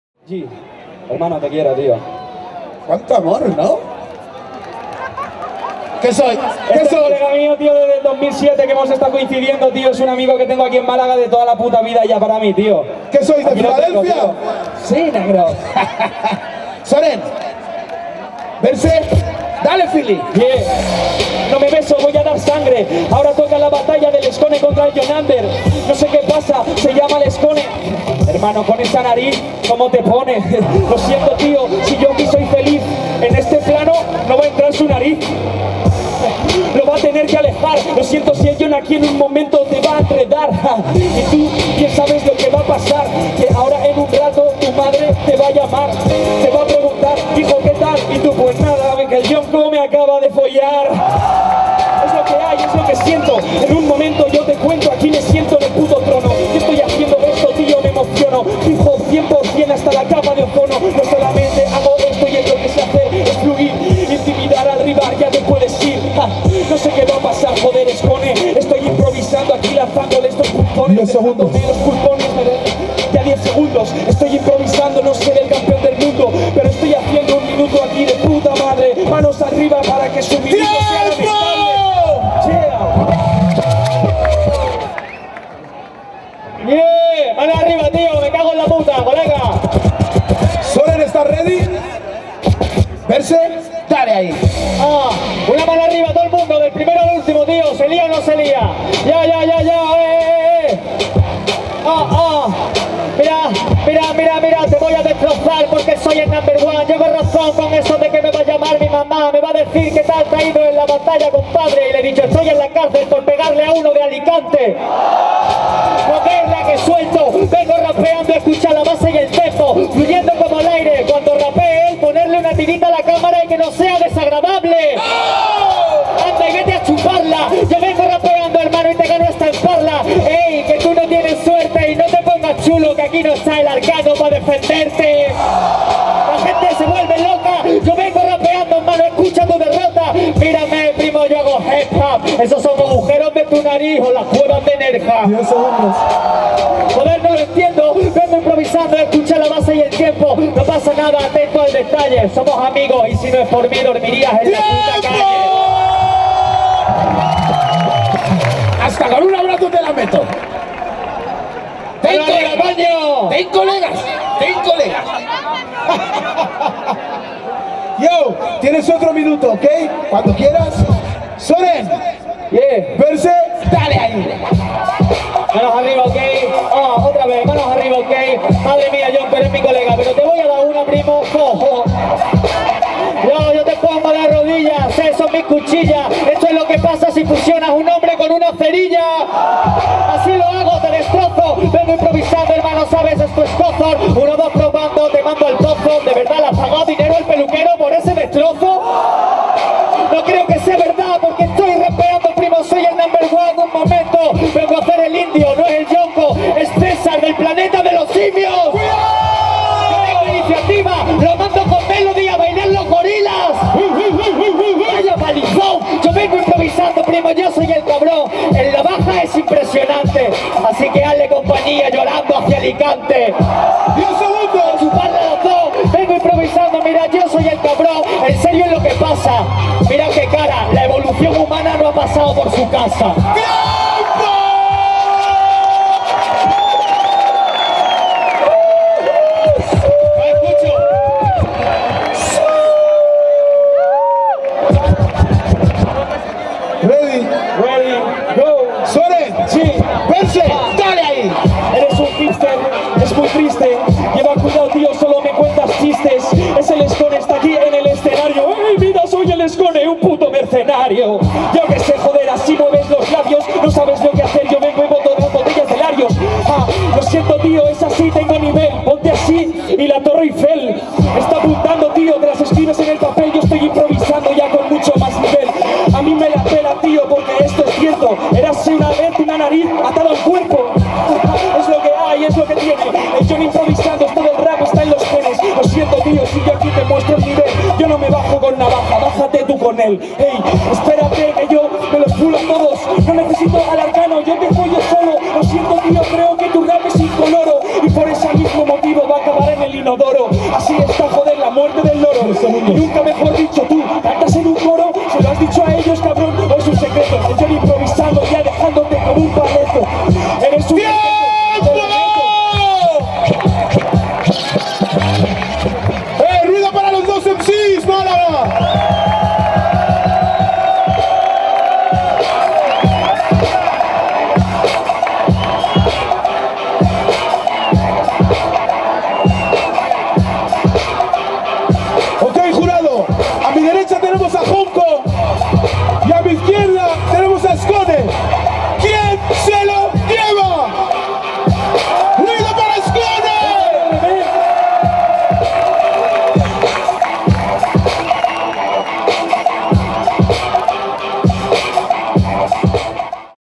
Skone VS Jonko - Cuartos - Batalla de los Gallos Málaga - Fresqueando
Skone VS Jonko.

Ganador: Skone.

Cuartos de final en Batalla de los gallos disputado en la capital de la costa del sol, Málaga.

Grabado/editado: David Garcia.